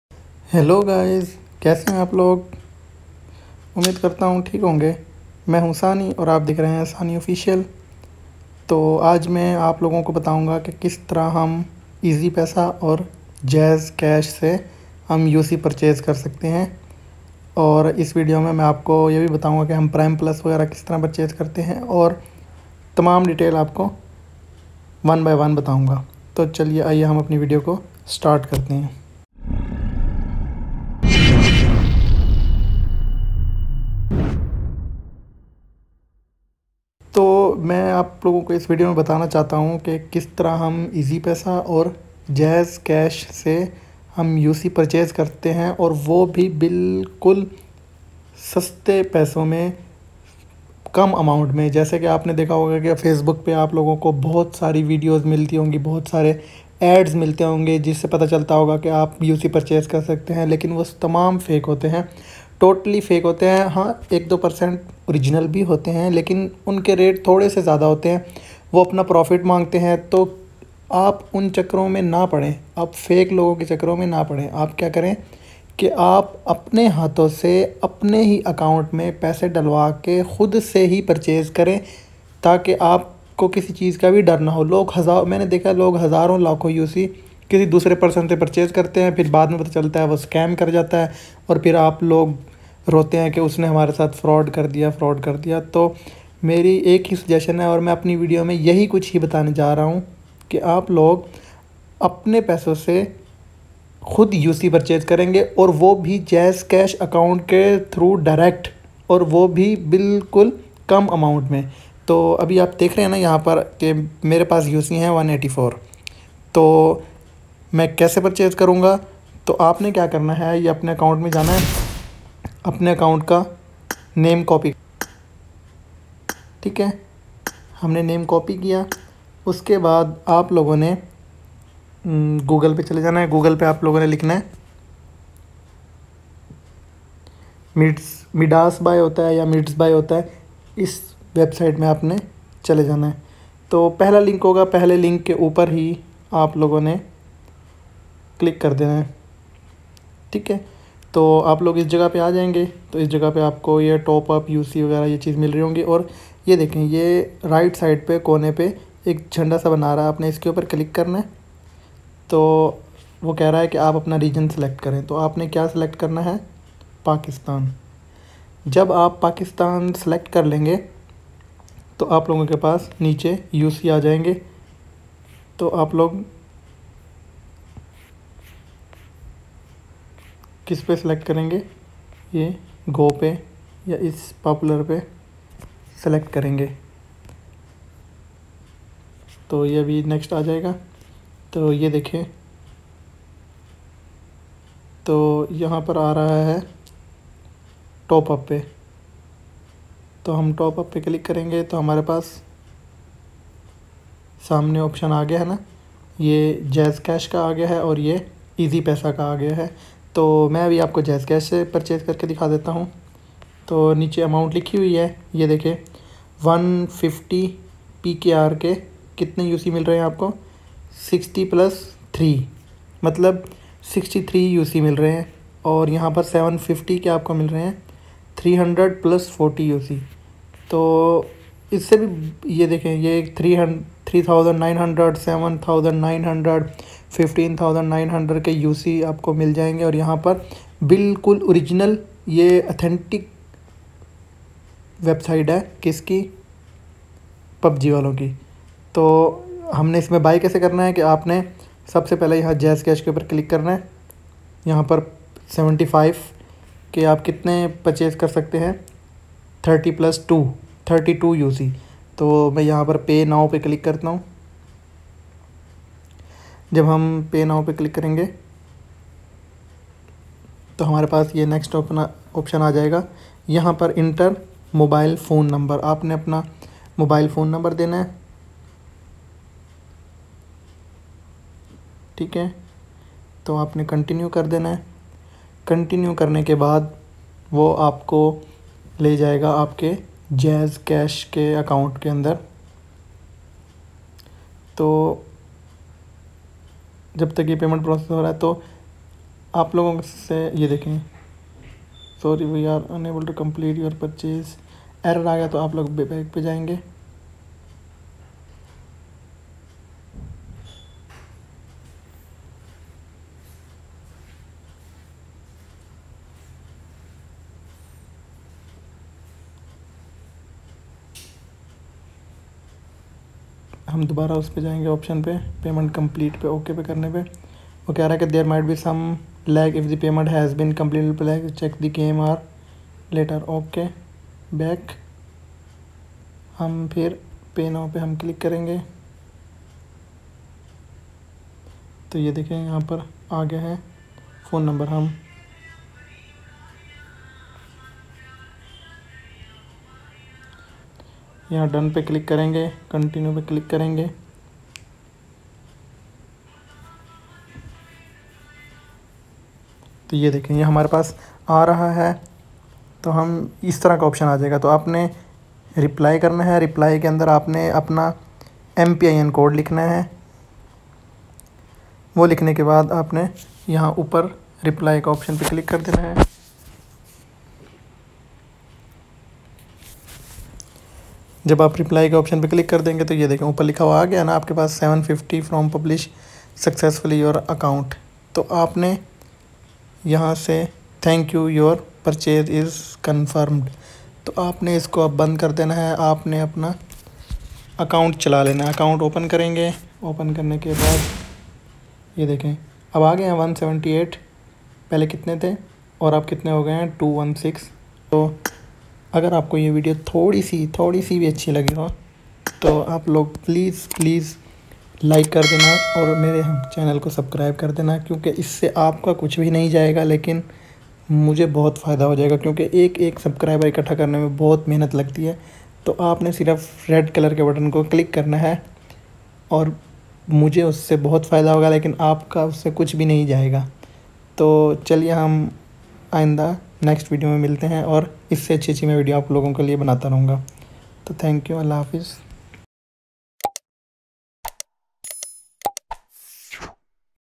How To Purchase UC Cheap Rates In Pakistan | SANI OFFICIAL
how to purchase uc in easypasa and jazzcash in our hand easy and simple method